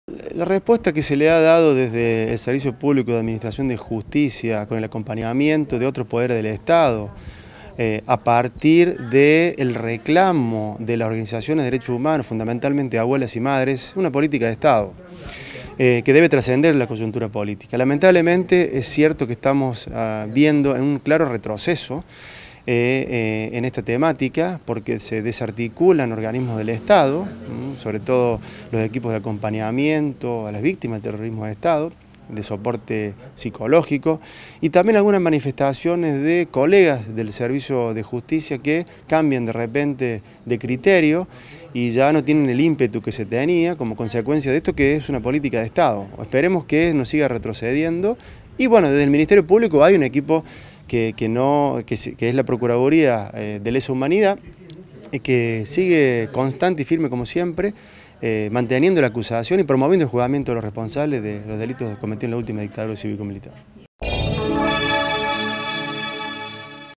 CAUSA POR DELITOS DE LESA HUMANIDAD
CARLOS GONELLA – FISCAL. REFLEXIONES SOBRE LAS ACTUALES POLÍTICAS EN DERECHOS HUMANOS.

Facebook.com/uniriotv
Twitter.com/uniriotv
Instagram.com/uniriotv
Web - uniriotv.unrc.edu.ar
o envianos un email a
UniRio TV
Universidad Nacional de Río Cuarto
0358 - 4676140